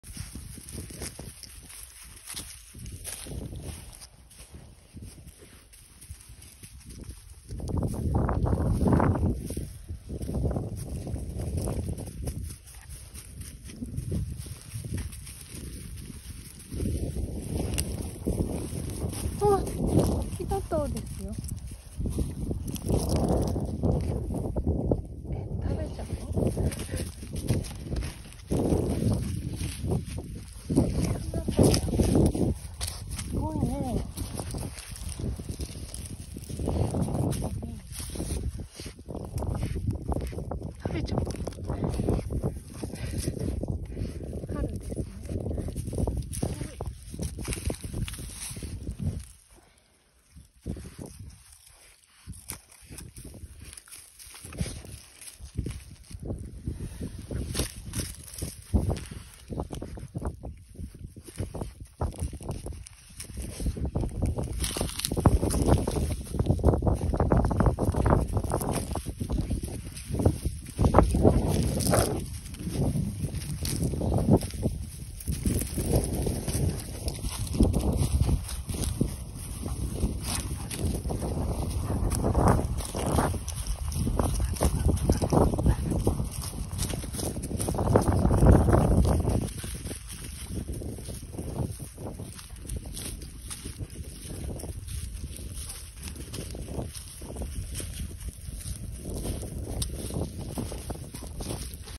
ふきのとうを見つけました
2021年3月31日
ふきのとうが出てきました。

はなちゃんとめいちゃんのお散歩中に、今年はじめてのふきのとうを見つけました。

きれいな色です。

春の訪れを感じますね。

*･゜ﾟ･*:.｡..｡.:*･*･゜ﾟ･*:.｡..｡.:*･⠀
保護した当日の様子 U^ェ^U⠀